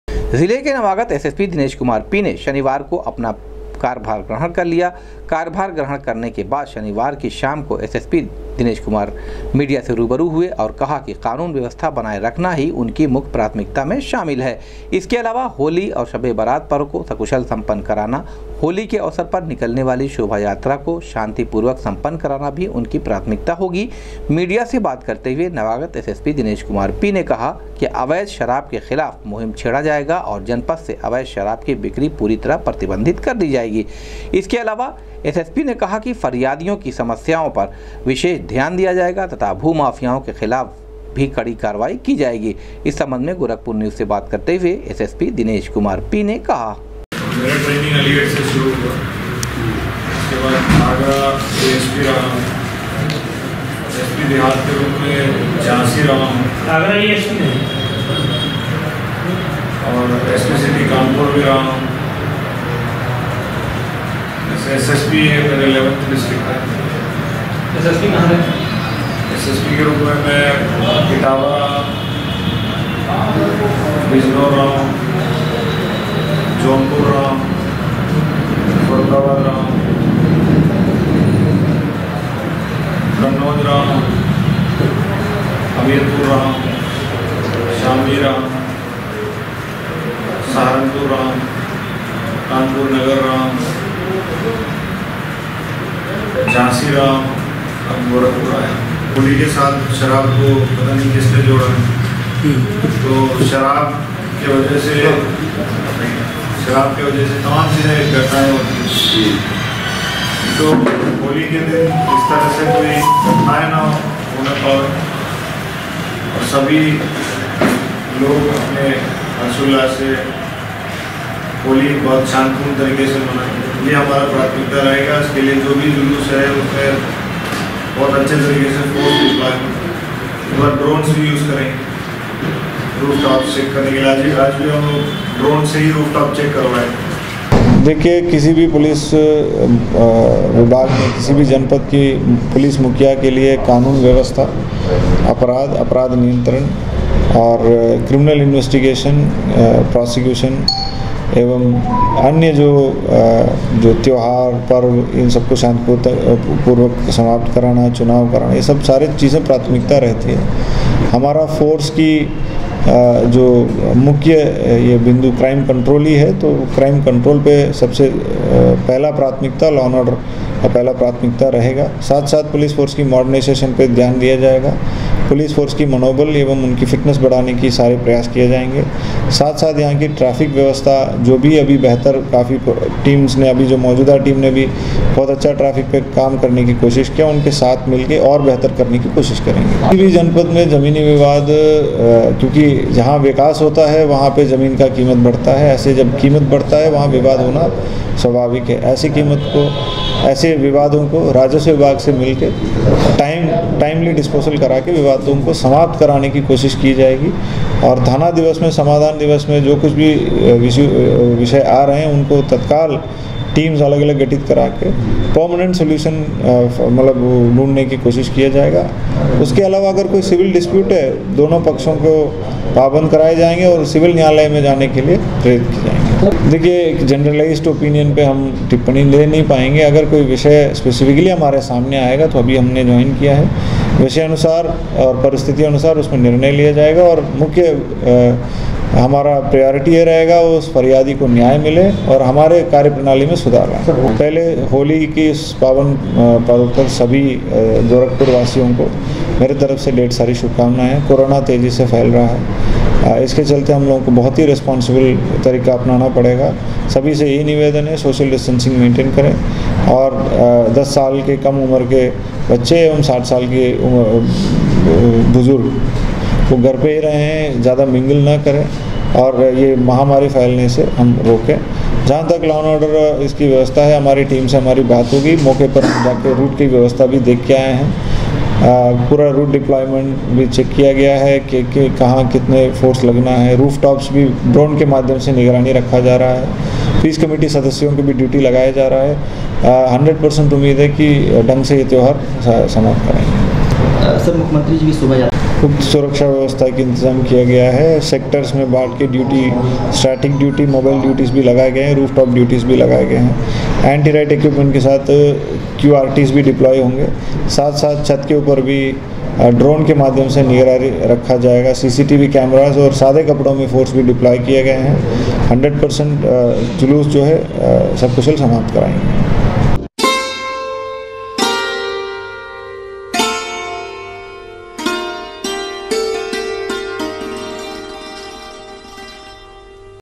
शनिवार को जिले की कमान संभालने के बाद नवागत एसएसपी दिनेश कुमार मीडिया से हुए रूबरू,कहा,कानून
शनिवार को जिले की कमान संभालने के बाद नवागत एसएसपी दिनेश कुमार मीडिया से हुए रूबरू,कहा,कानून व्यवस्था बेहतर बनाना ही होगी उनकी मुख्य प्राथमिकता